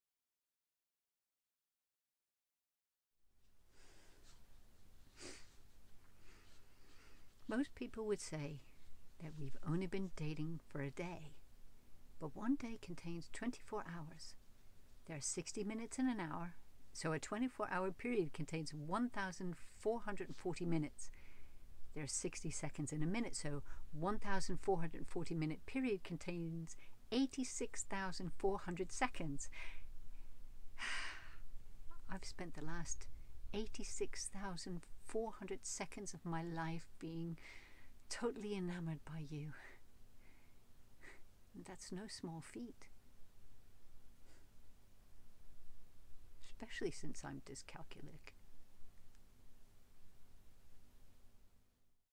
Kelley Costigan - One Day Anniversary (Day 13)
The Acting Habit 21-Day Self-Tape Callback – Spring 2022 Edition (Manuel Puro). 21 days of scripts for showreels and scene sharing.

Ever the romantic, even from day one!